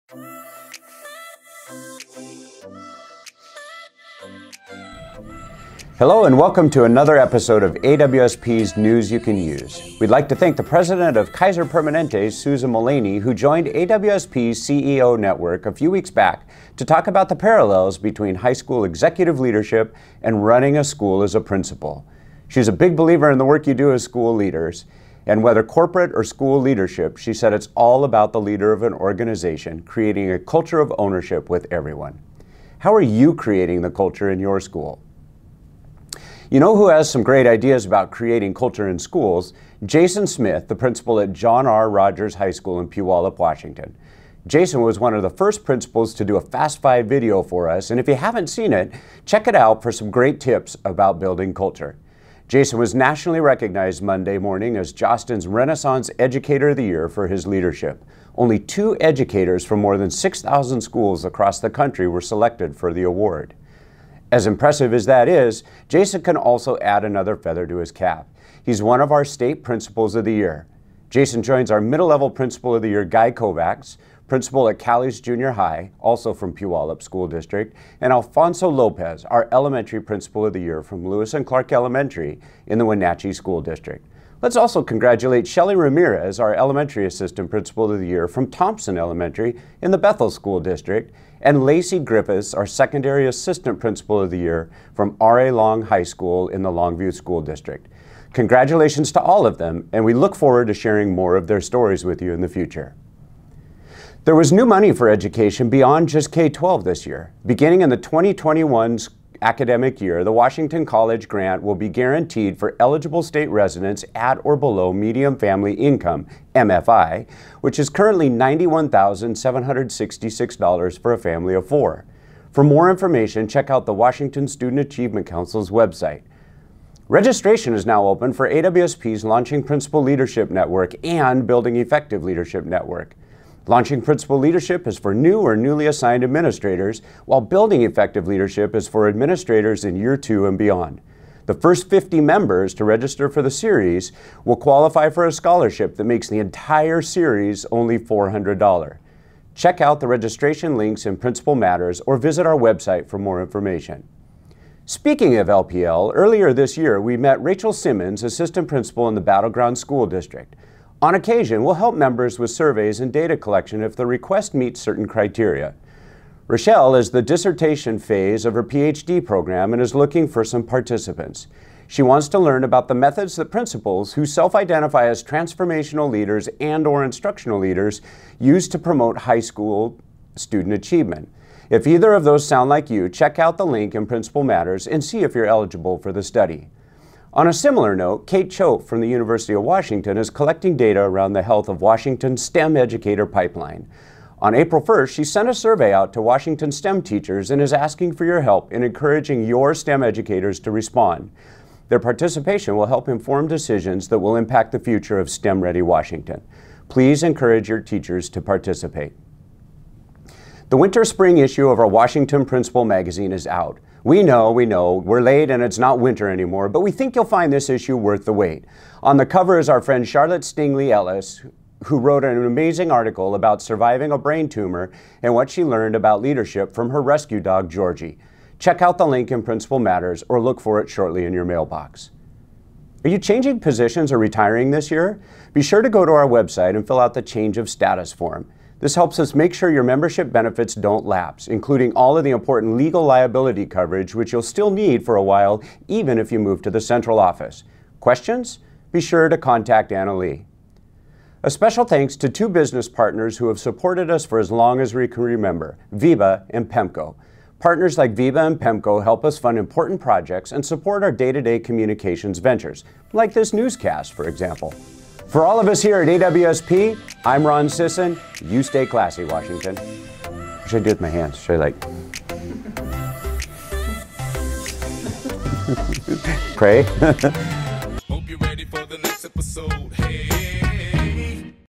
AWSP News for May 15, 2019
In this episode of AWSP News, we discuss a visit from Kaiser Permanente Susan Mullaney, principal Jason Smith from John R. Rogers High School in Puyallup and his national recognition, our state principals of the year, the Washington College Grant, registration for AWSP’s Launching Principal Leadership Network AND Building Effective Leadership Network, a Ph.D. candidate wanting to learn more about principals as transformational and instructional leaders, a survey on Washington’s STEM educator pipeline, the winter/spring issue of our Washington Principal magazine, and our business partners VEBA and PEMCO.